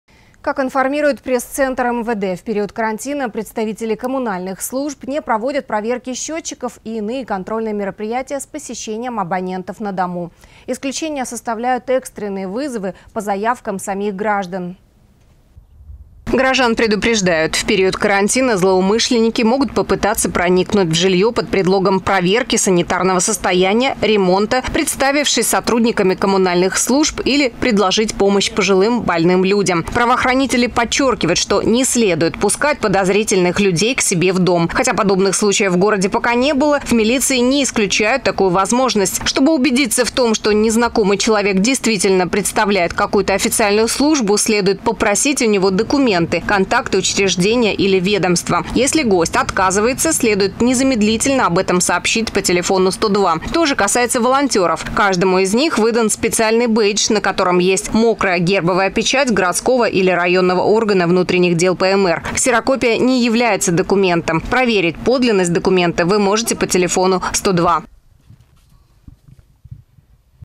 Не терять бдительность
Как информирует пресс-центр МВД, в период карантина представители коммунальных служб не проводят проверки счётчиков и иные контрольные мероприятия с посещением абонентов на дому. Исключение составляют экстренные вызовы по заявкам самих граждан.